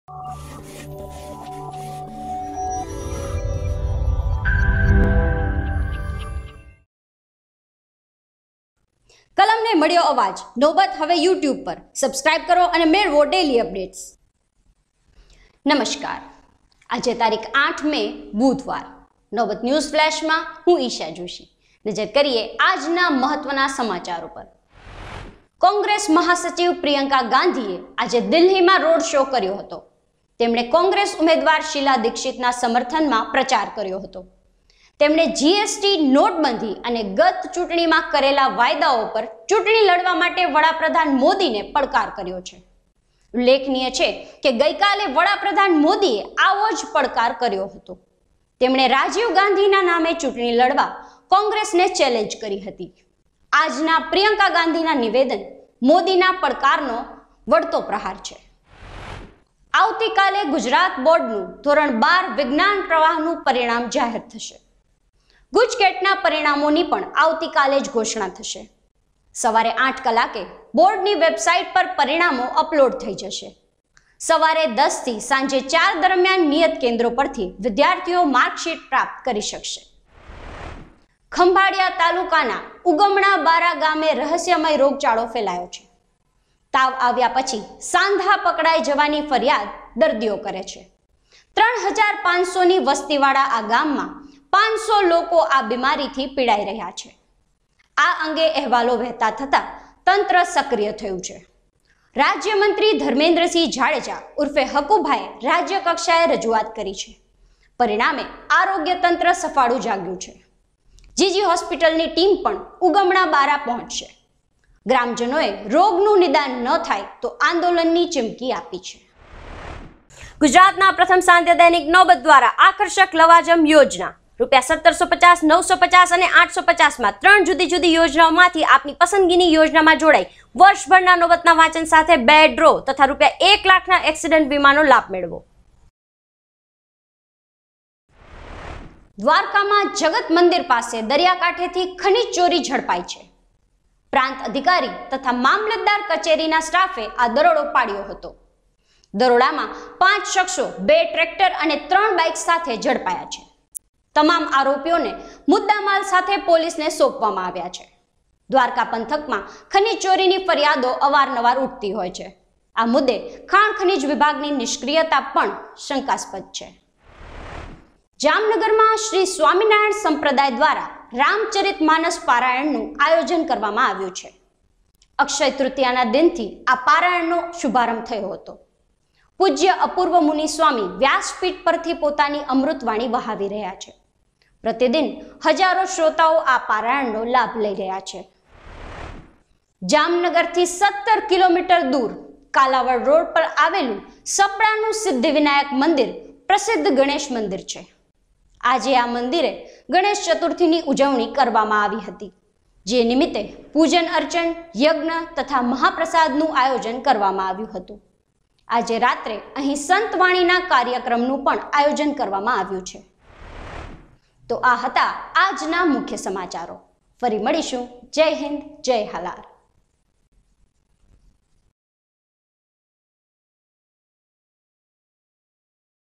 News Flash: Priyanka Gandhi Challenged PM Modi Over GST & Note Ban
1. Priyanka Gandhi Challenged PM Modi: Fight Elections with GST, Note Ban & Incomplete Promises.
2. 12th Science & GujCET Results will be Declared Tomorrow.
3. 500 Villagers are suffering from a Suspicious Disease in Ugamanabara village.
4. 4 thives caught Red Handed from Dwarka Costal Area in mineralogy case.
5. ‘Ramcharitmanasa’ by Swaminarayan Community - Jamangar.
6. Ganesh Chaturthi Celebration in Sapada, Jamnagar.